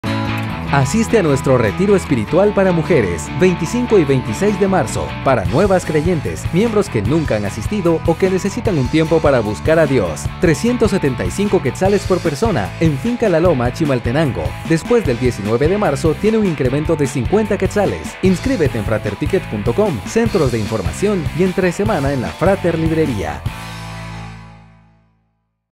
Retiro de mujeres marzo
Mujeres, asistan al retiro de 2 días donde podrán renovar sus fuerzas, reconciliarse con Dios y consigo mismas. Se llevará a cabo el 25 y 26 de marzo en Finca La Loma, Chimaltenango.

El valor es de Q375 por persona, después del 29 de enero tiene un incremento de Q50. los Centros de Información o entre semana en Fráter Librería.